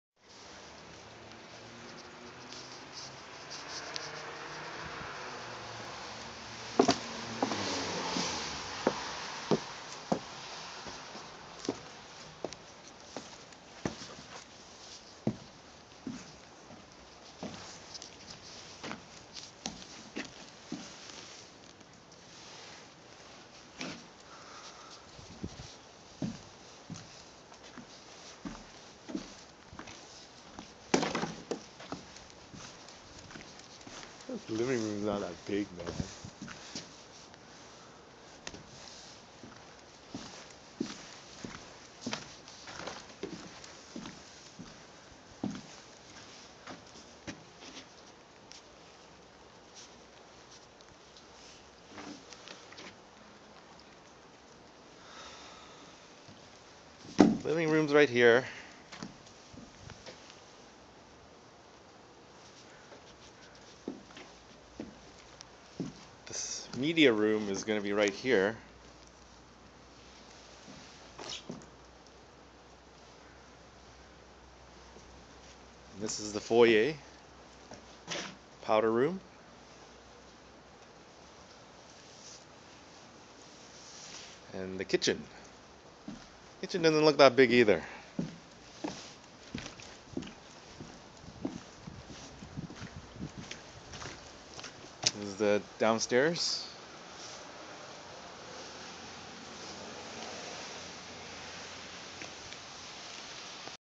Framing, part I
Most of the first floor framing is complete.